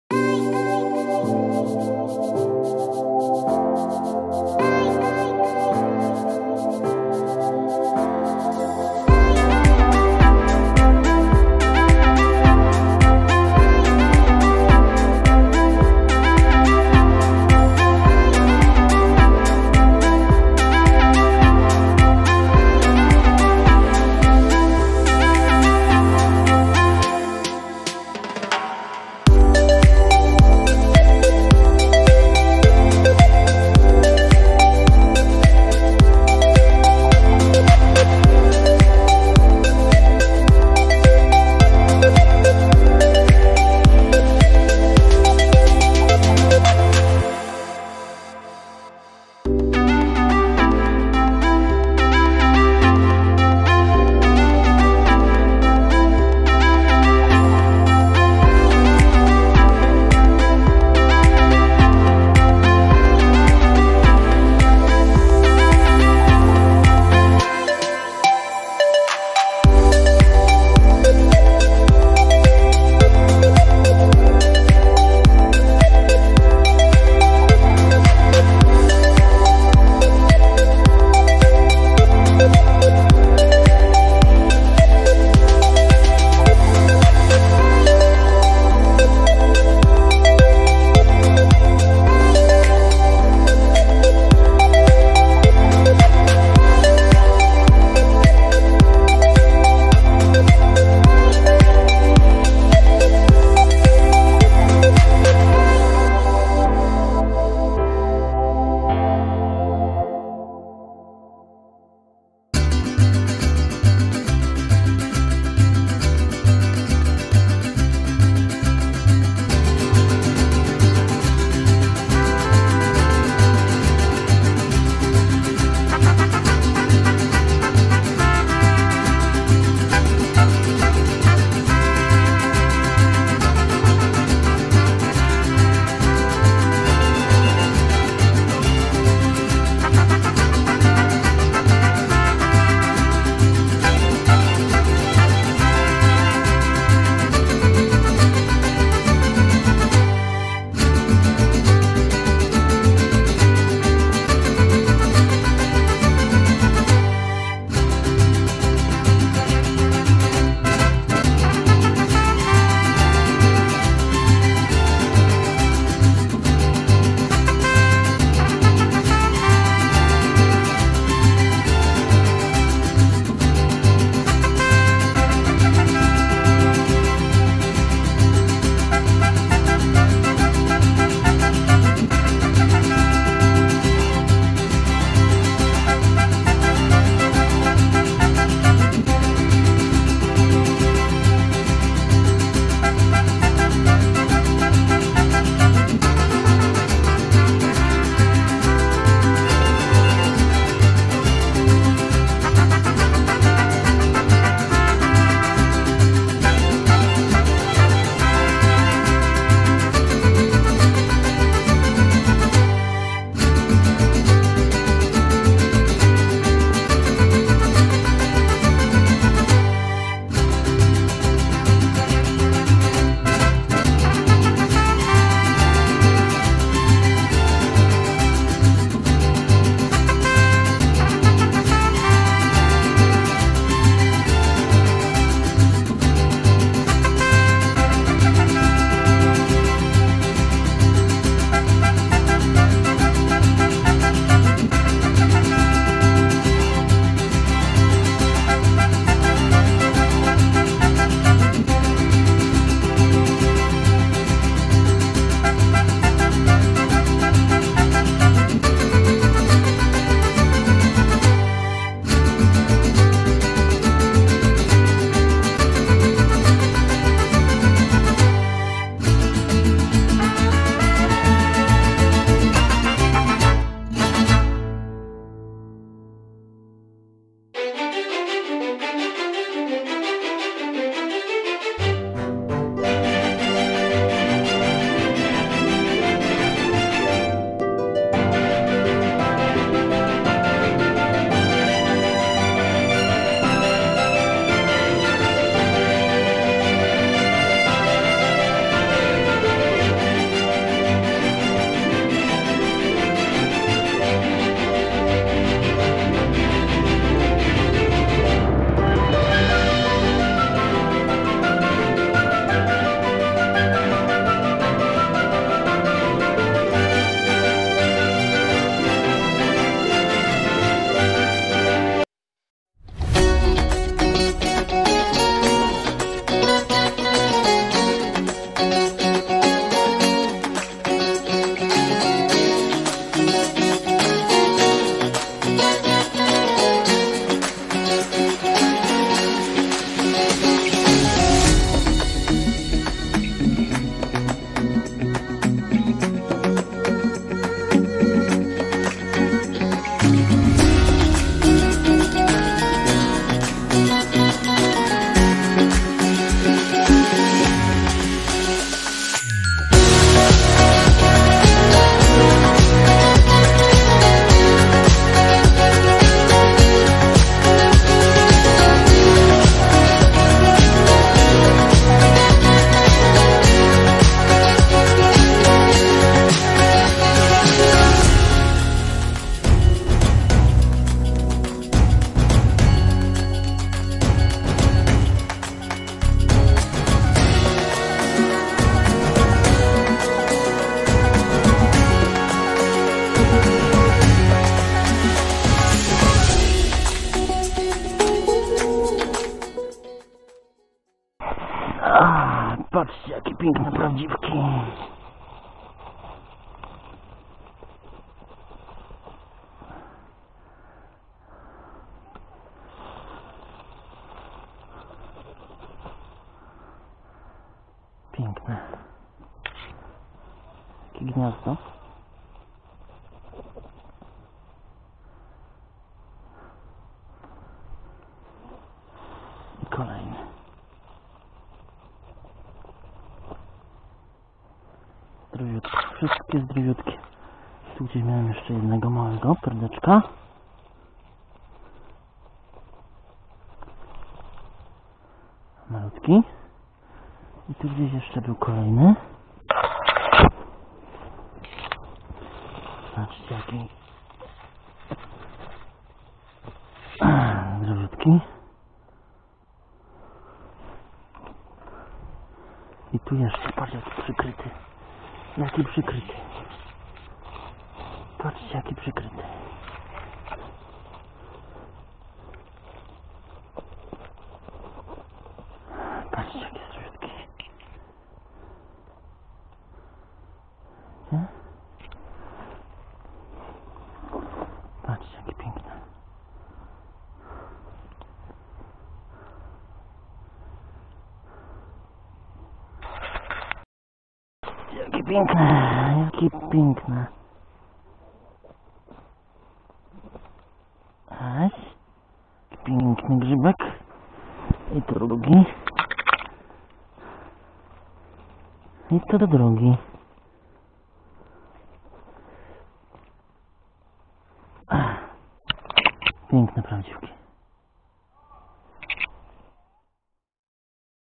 Grzybobranie 22.09.2019 | "Koszenie" maślaków-,prawdziwków.
Wreszcie u nas są grzyby a już wkradało się zwątpienie.
Grzyby są ale to mało powiedziane jest ich Bardzo Duuuuużo.